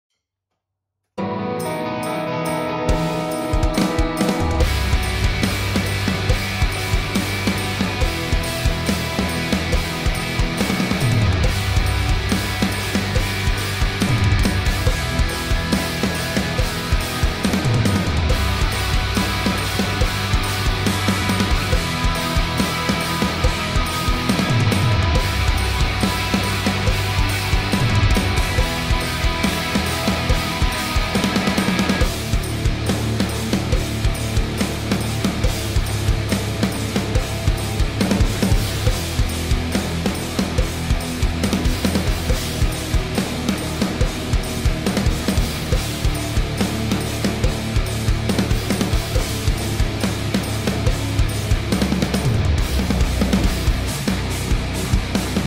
Get Good Drums Roland TD-17 KVX Demo
This is a demo of Get Good Drums using the PIV + Modern & Massive mixed together.

Synth - Matt Palmer
Guitar - Adam James
Drums - Adam Garner

Equipment

GGD Modern & Massive
GGD PIV
Roland TD-17 KVX